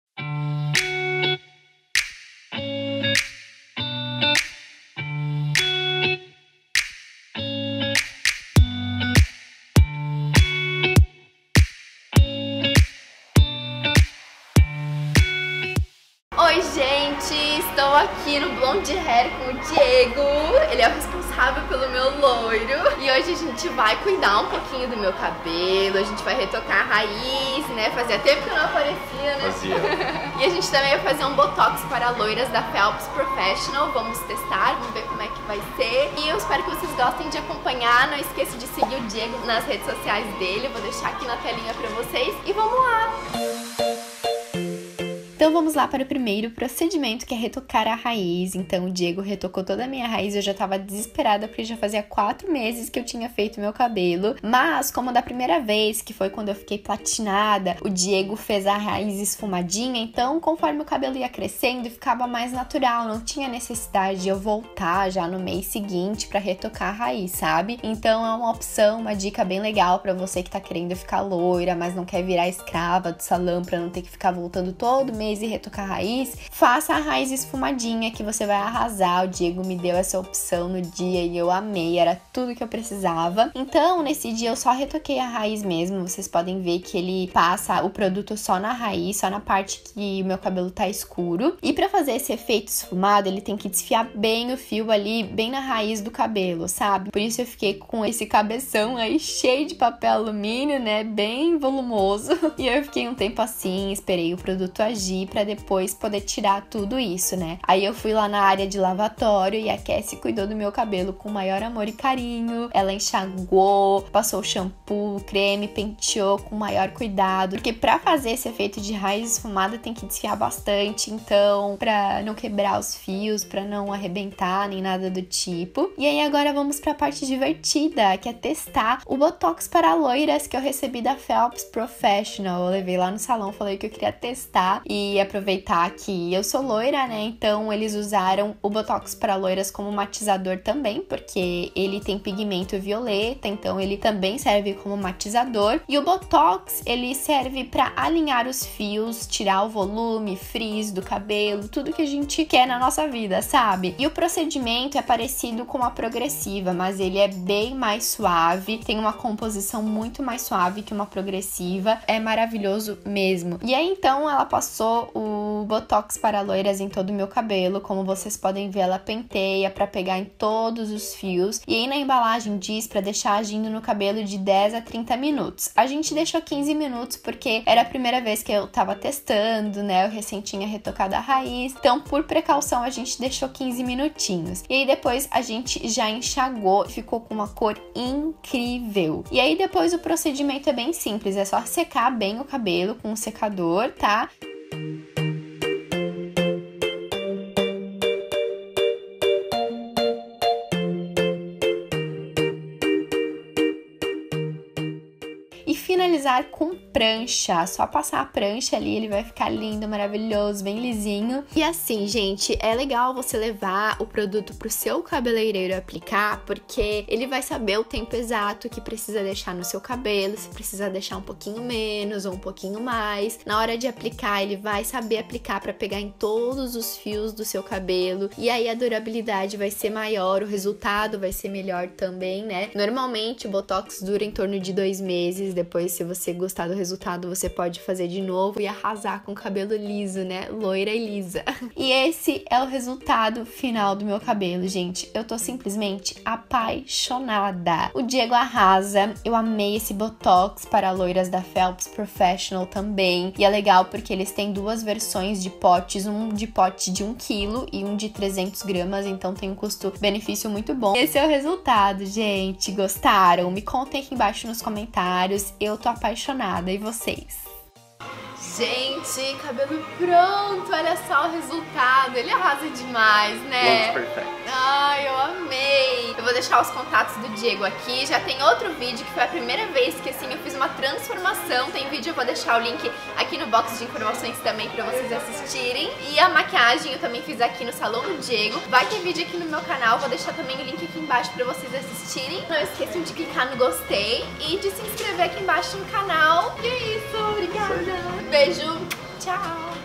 COMO TER O CABELO LOIRO E LISO DOS SONHOS | Jana Taffarel
Chegou o dia de retocar a raiz e ficar com o Blond Perfect! O Diego e equipe cuidaram do meu cabelo com o maior amor e carinho e também fizemos o Botox para Loiras da Felps Profissional que deixou aquele loiro dos sonhos, sabe? Vem ver todo o processo e o resultado final!

Caixa Postal:
Janaina Taffarel
Rua 600 n 111 Caixa Postal 1070, Centro
Balneário Camboriú/SC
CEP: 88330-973